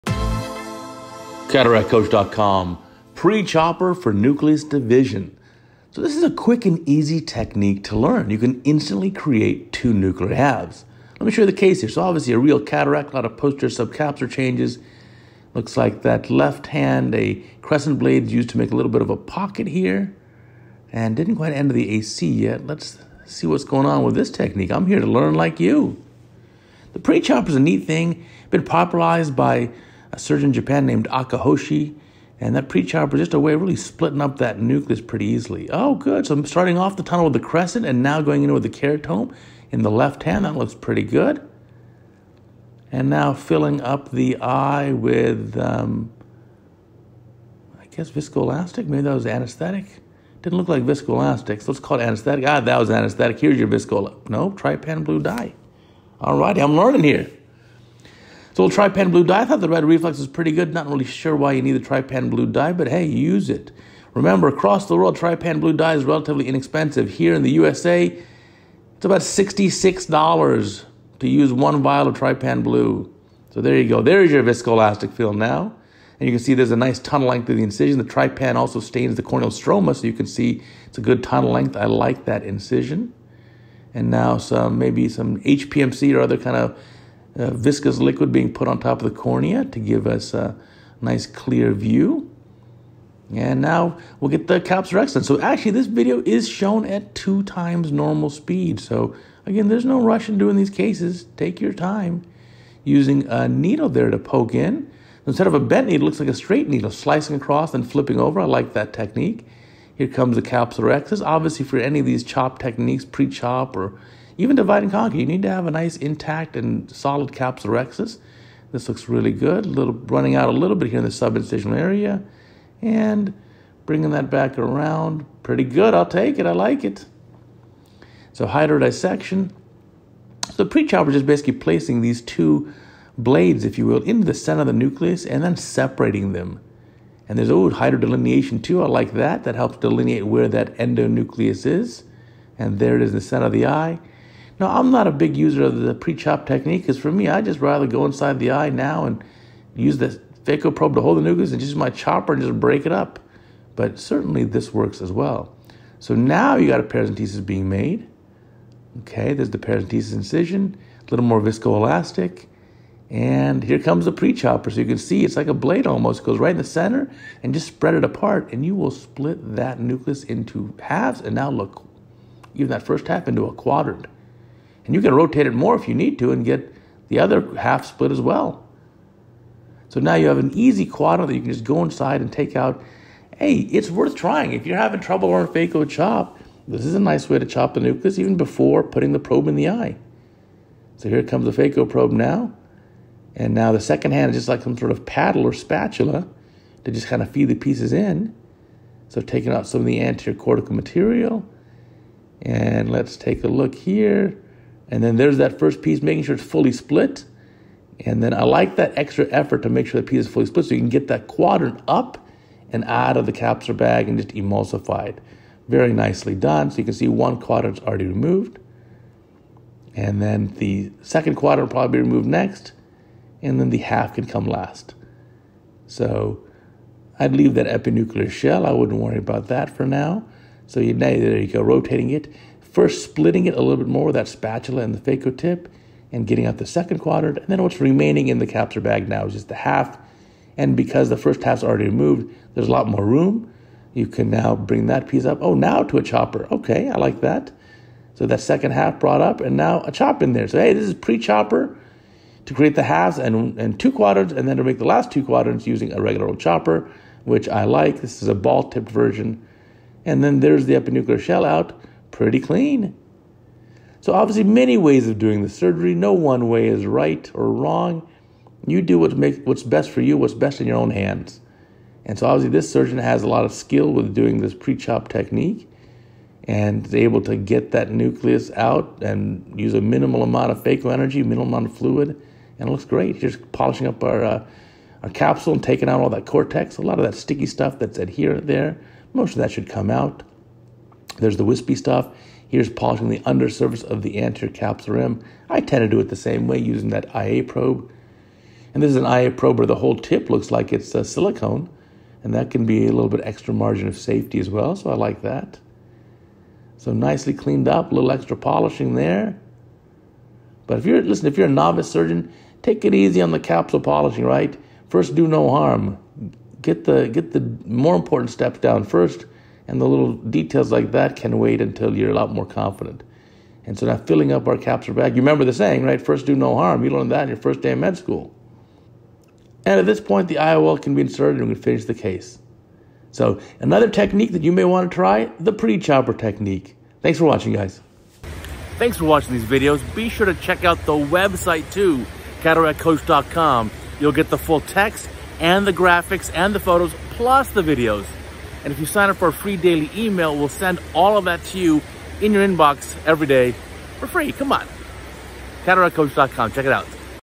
CataractCoach 1333: pre-chopper for nucleus division in cataract surgery
The pre-chopper is an instrument with two blades which are shaped like paddles and they are embedded into the nucleus and then pushed apart in order to divide the cataract into smaller pieces. It is a quick and easy technique to learn and it was popularized by Dr Akahoshi from Japan. It certainly works well in this surgeon's hands and I encourage you to try it if you are struggling to learn phaco chop.